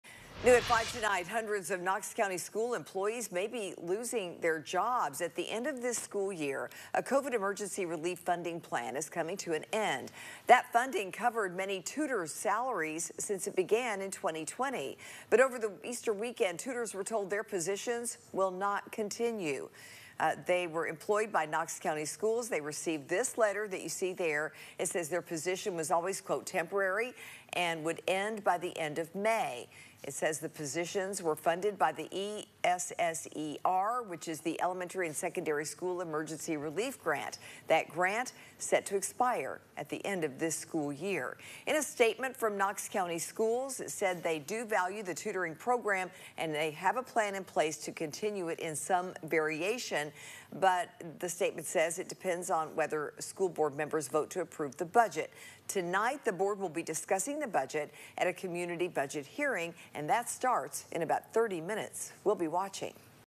Knox County Schools working to provide financing for tutors after COVID-era program expires
A COVID emergency relief funding plan that provided funding the salaries of many tutors for Knox County Schools is set to expire at the end of this school year. District officials are exploring alternate financing plans to keep tutors employed.